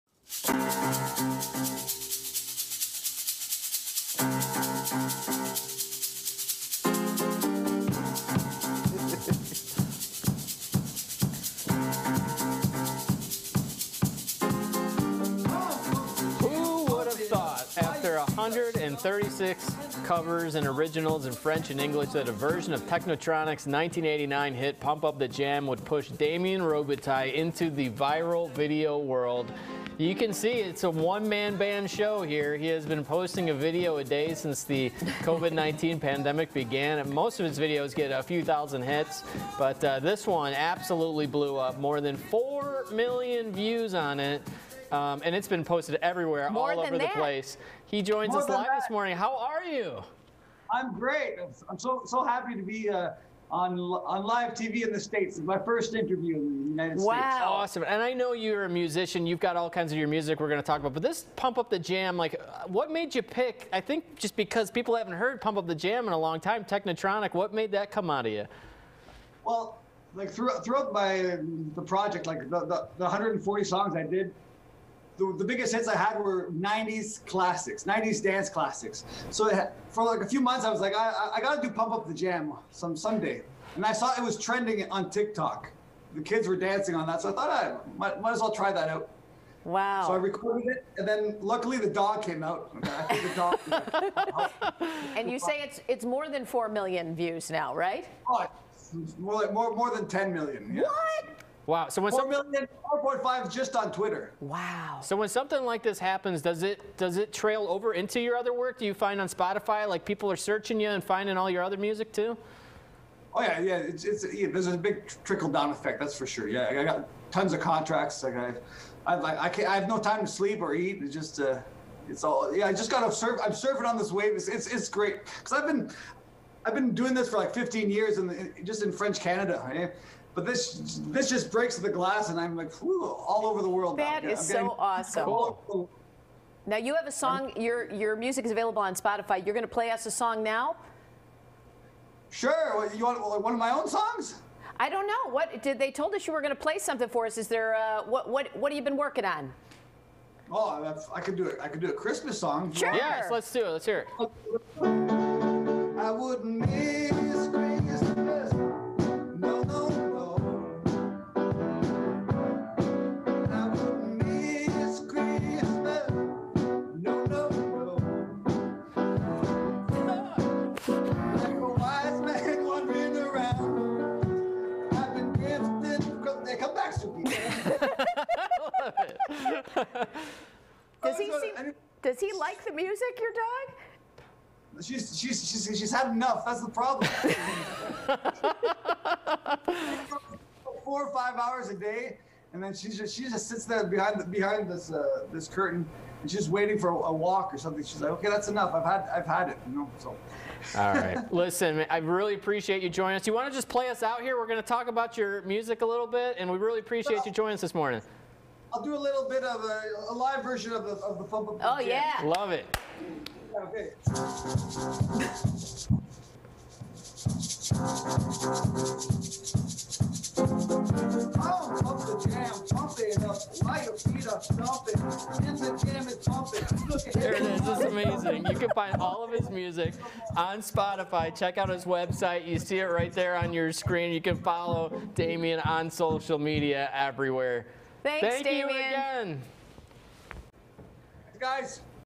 Damien Robitaille's one-man band of cover tunes has made him a viral video star!
His daily one-man band of cover tunes has made him a big internet star and is opening the door to his original music!